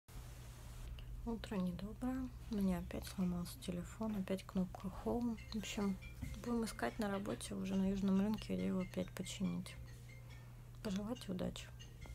Утро добрым не бывает. IPhone Se снова не работает кнопка HOME.
Вот везёт же мне ... нет слов